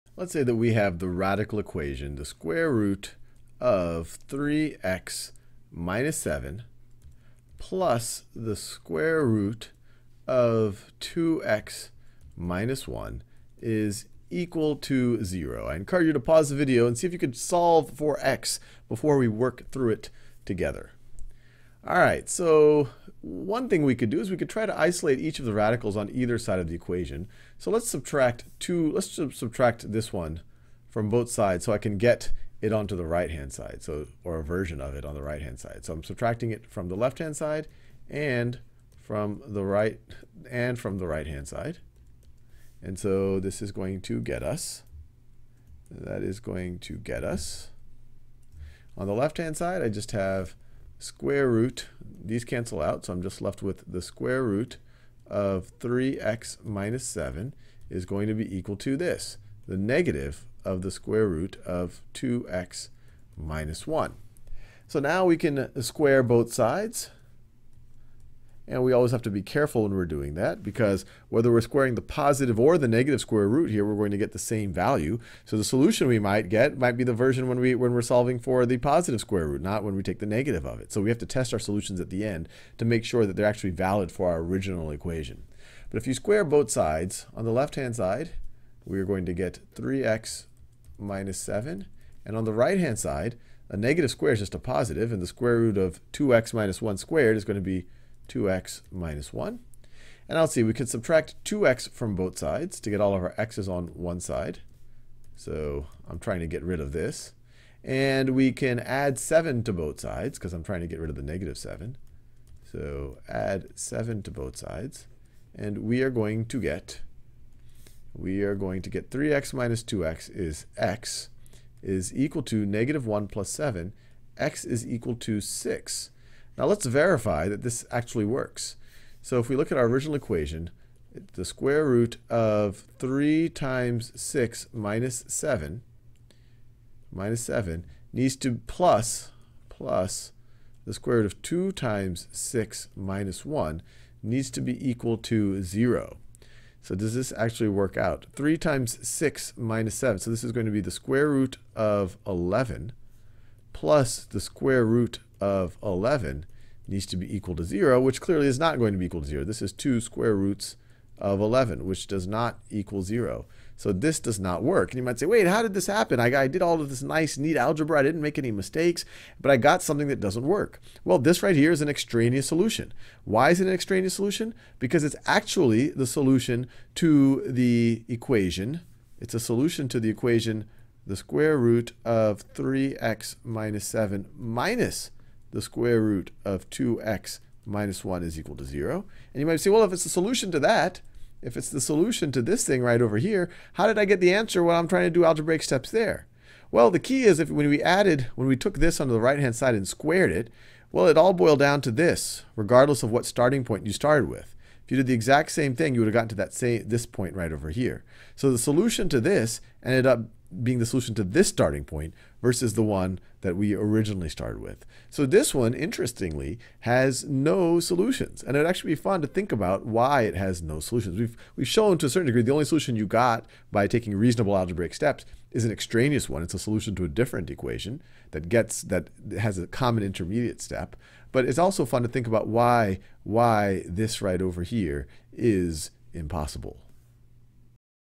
Solving square-root equations: no solution | Mathematics III | High School Math | Khan Academy
Courses on Khan Academy are always 100% free. Start practicing—and saving your progress—now

Sal solves the equation √(3x-7)+√(2x-1)=0, only to find out that the single solution is extraneous, which means the equation has no solution.